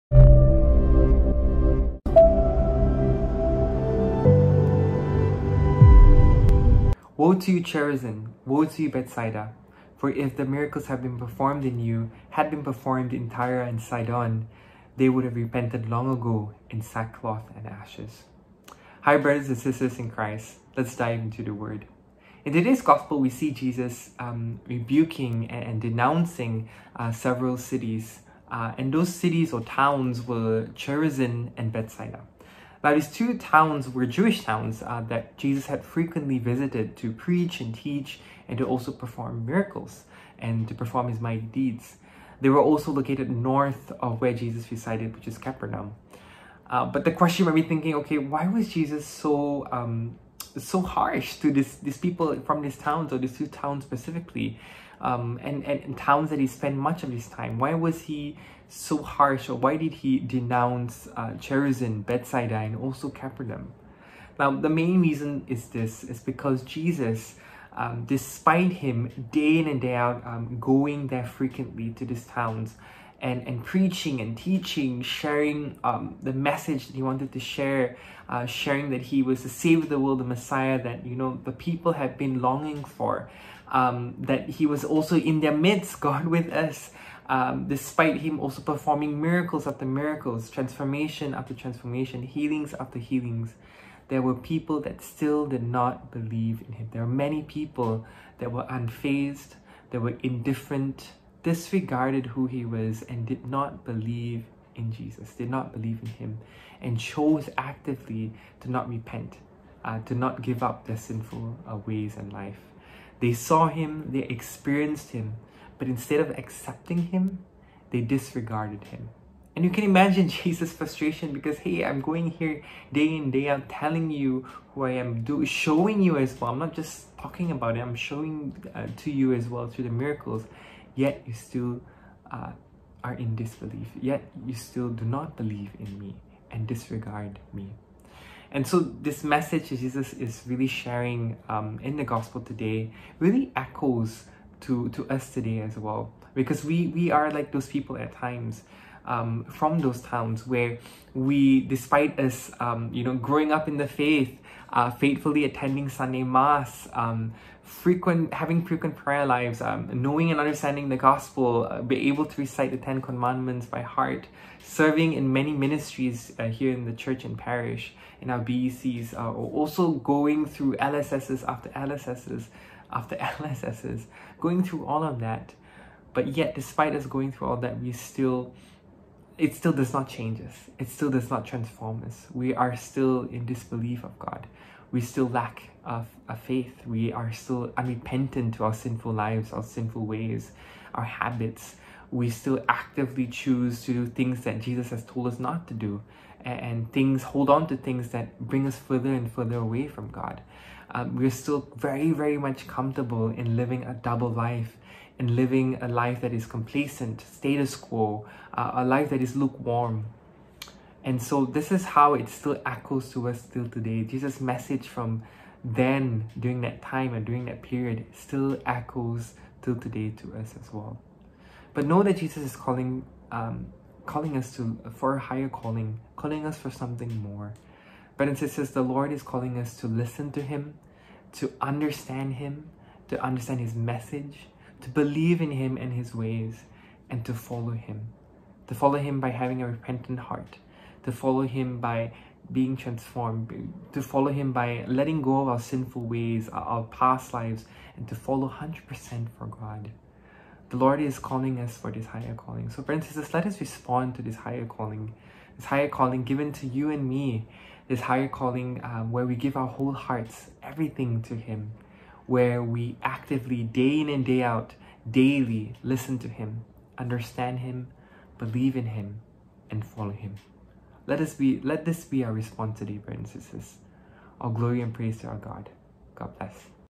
'ANSWERING GOD'S HIGHER CALLING' by John Paul. INTO THE WORD: Tuesday 12/7/22
INTO THE WORD is a ministry that gives short daily reflections on the day's readings of the Catholic Church. Today’s reflection is given by John Paul Felix a young adult from Subang Jaya, Selangor, Malaysia. He has a Masters in Managerial Psychology and currently works in KL. He went to the Philippines in 2017 and was a missionary there for 10 months.